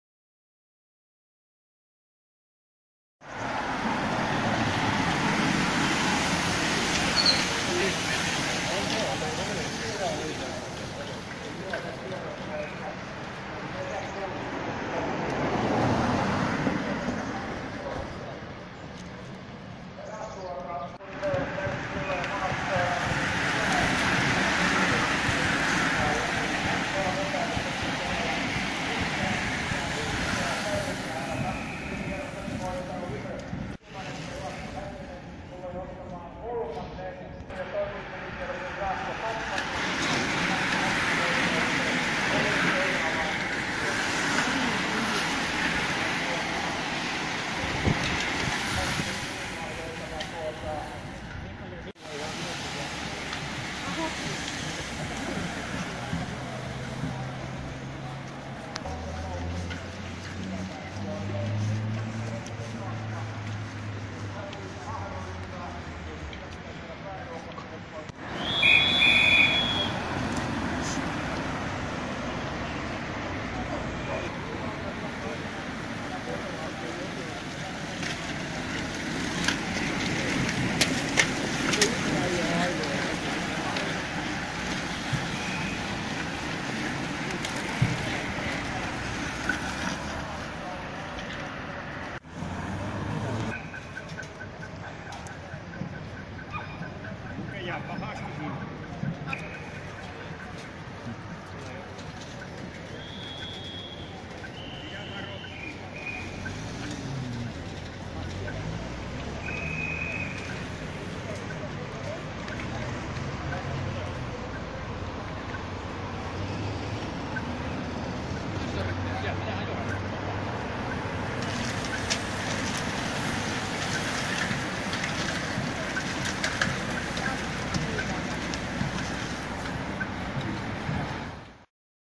TS kortteliajot 24 4 2016, Turku
TS kortteliajot cycling race in Turku, Finland.